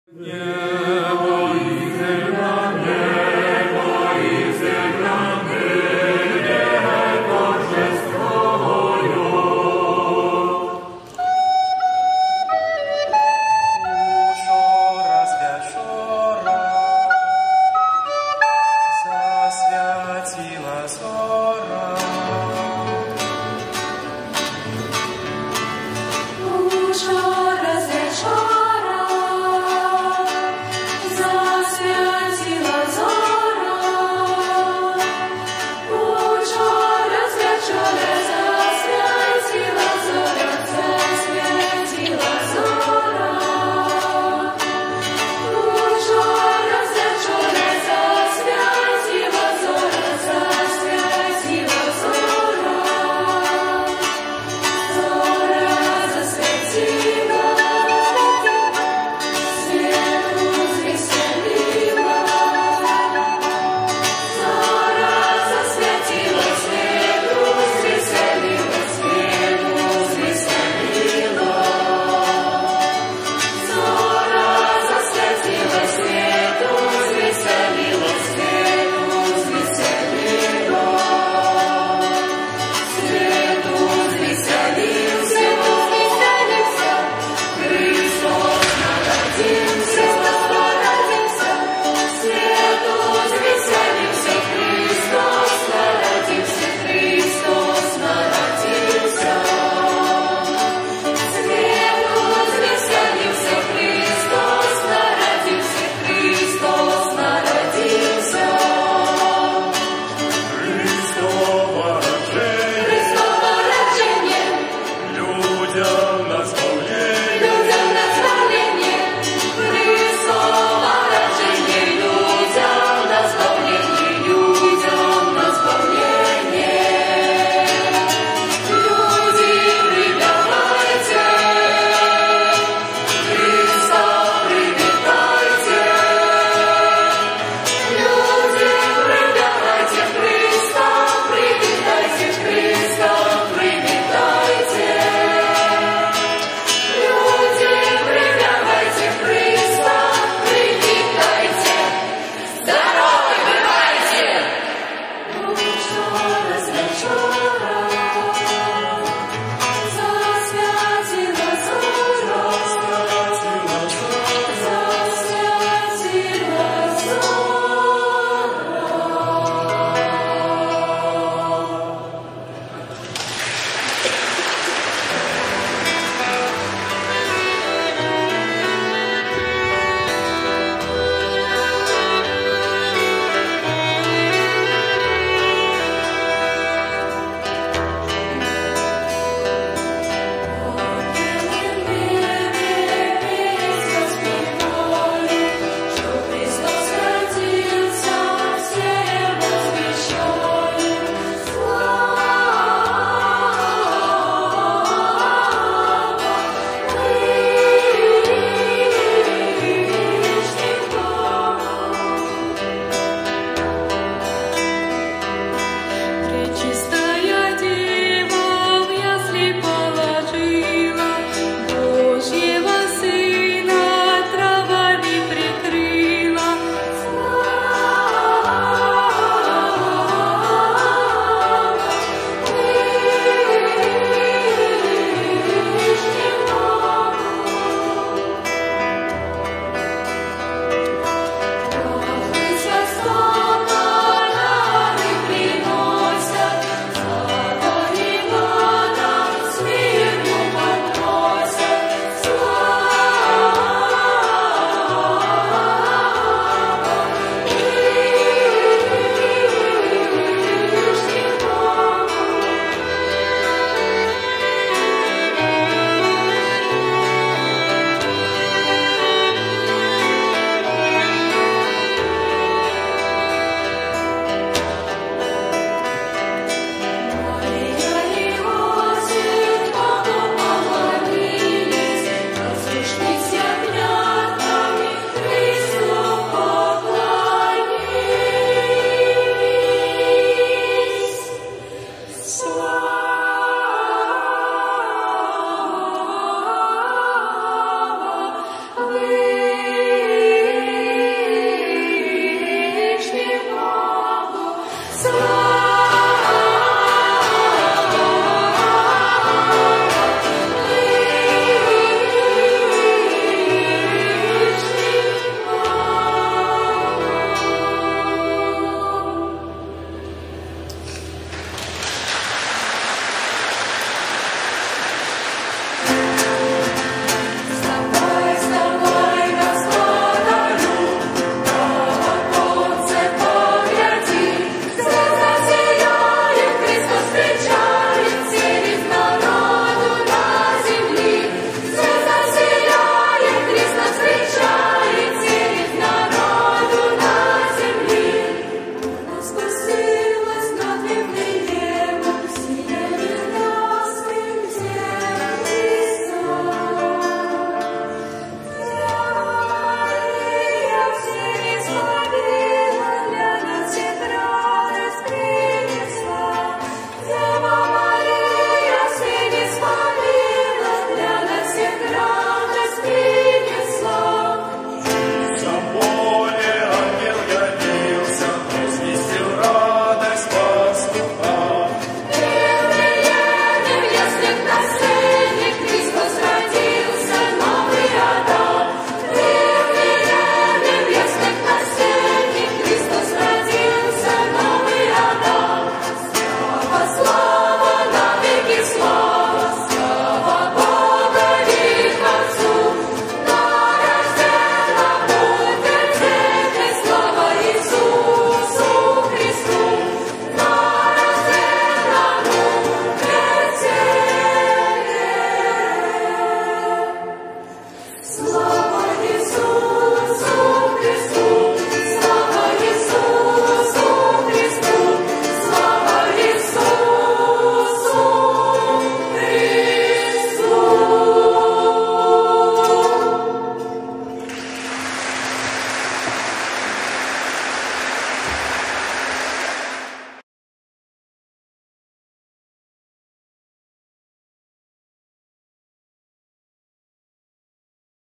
XXV MFKW - Chór „Wstrecza” z Mińska na Białorusi
Dziewiątym wykonawcą w sobotę pierwszego dnia Jubileuszowego XXV Międzynarodowego Festiwalu Kolęd Wschodniosłowiańskich był Chór Dziecięco – Młodzieżowy „Wstrecza” Parafii Prawosławnej Ikony Matki Bożej „Wszystkich Strapionych Radość” z Mińska na Białorusi. Koncert odbył się 25 stycznia 2020 roku w Miejskim Ośrodku Kultury w Terespolu.